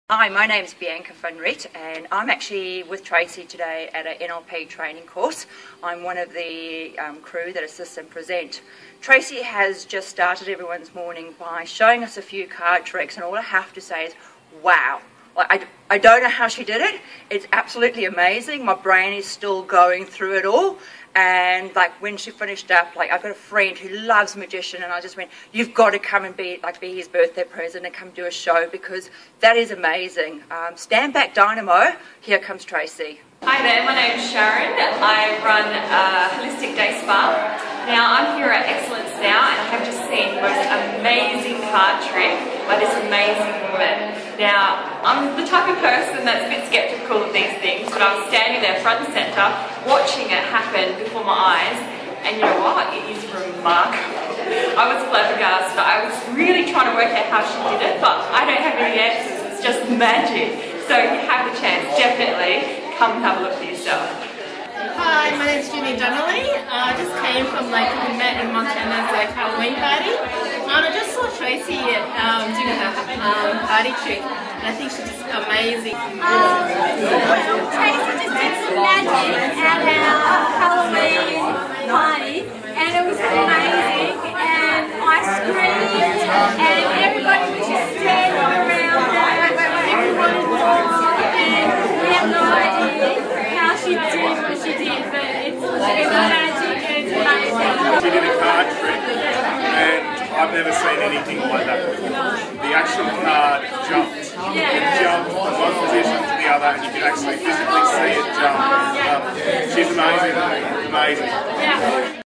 Trace of Magic Sydney Magician | Corporate Workshop & Entertainment for Live it Now REVIEW
Trace of Magic (Magician Trace) is one of Sydney's premier corporate and private party comedy magician. Listen to what client has to say about the entertainment...

Don't hesitate to add me...

Magician Trace is a cheeky and amazing comedy magician. Illusionist magic event planner planning sydney corporate entertainment private party entertainment reviews